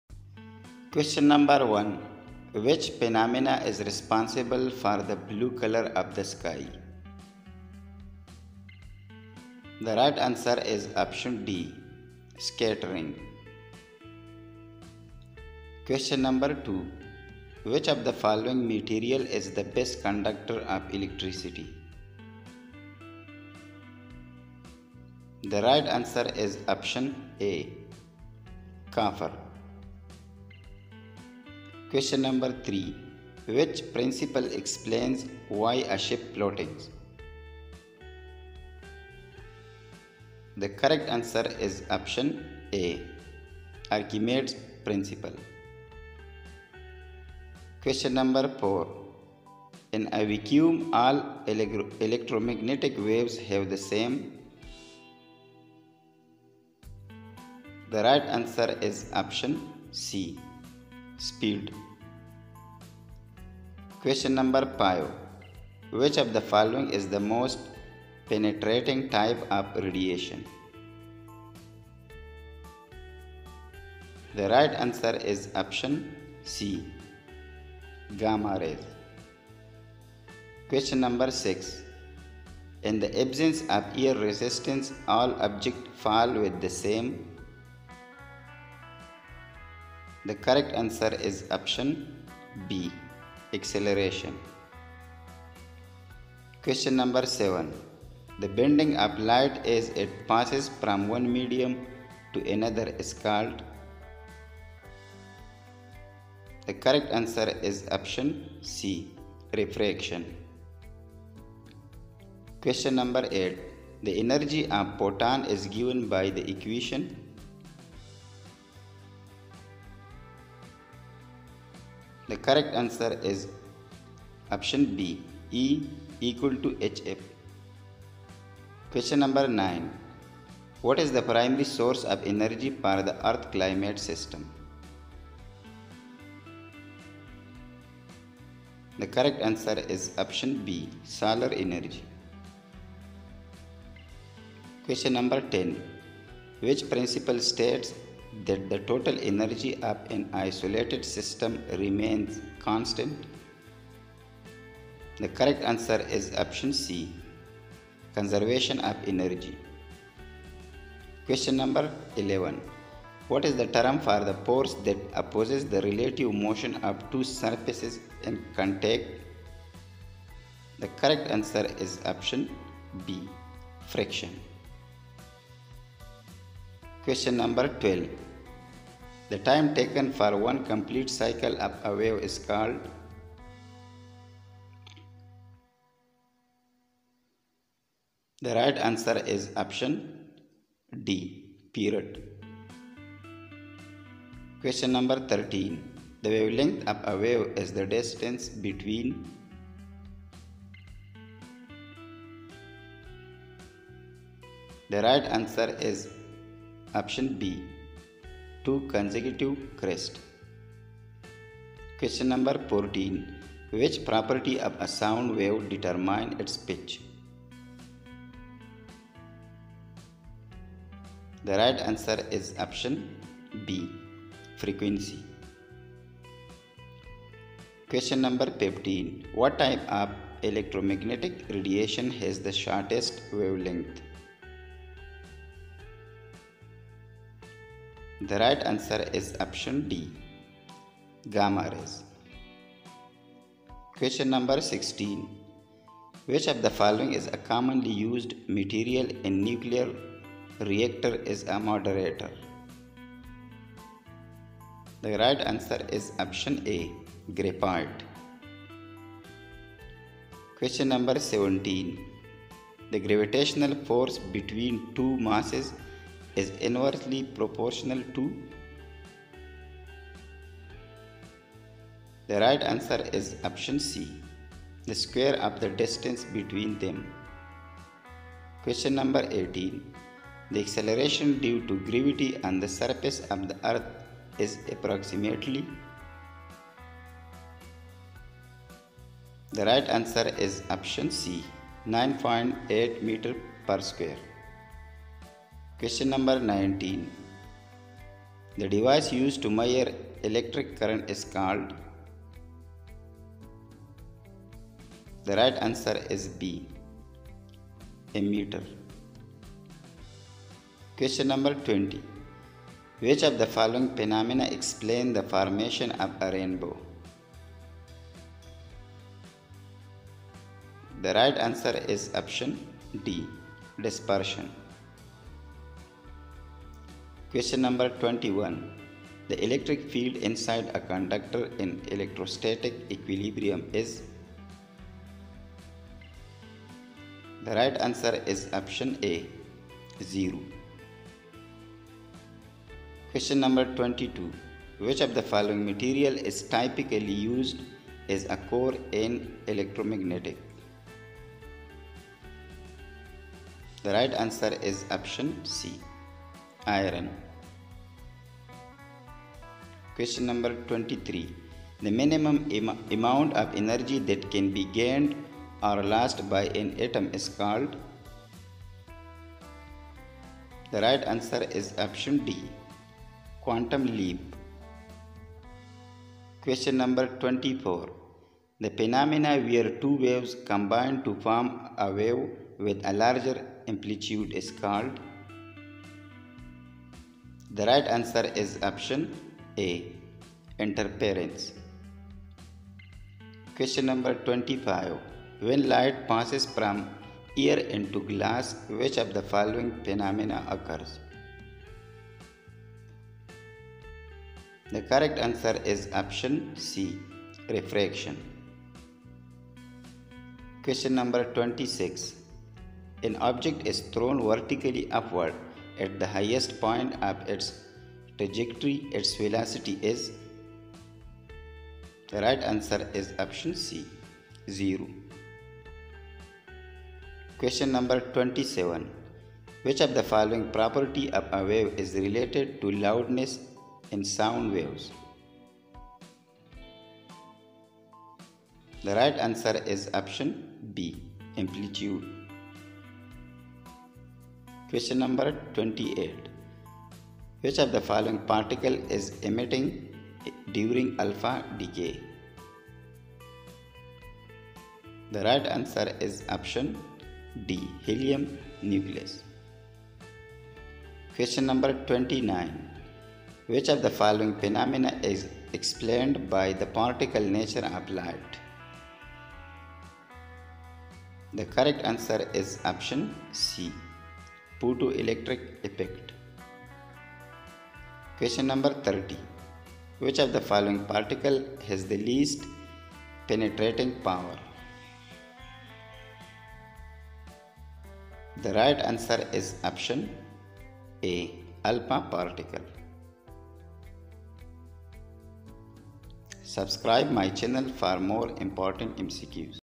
Physics MCQs | Test your knowledge
In this video, we'll be covering a range of physics topics, from motion and forces to energy and electromagnetism. Whether you're a student looking to review for a test or a science enthusiast wanting to challenge yourself, this video is for you!

Topics Covered:

Kinematics and dynamics
Energy and work
Momentum and collisions
Electricity and magnetism
Waves and optics

MCQs:

Q1: Which phenomenon is responsible for blue colour of the sky?

a) Dispersion
b) Reflection
c) Refraction
d) Scattering
Right answer is, d) Scattering

Q2: Which of the following materials is the best conductor of electricity?

a) Copper
b) wood
c) Glass
d) Rubber
correct answer is, a) Copper

top 30 physics mcq
physics mcqs for pgcet exam
pgcet entrance exam mcq
physics mcq for mdcat
physics mcq for sst test

_Comment_ below with any questions or topics you'd like us to cover in our next video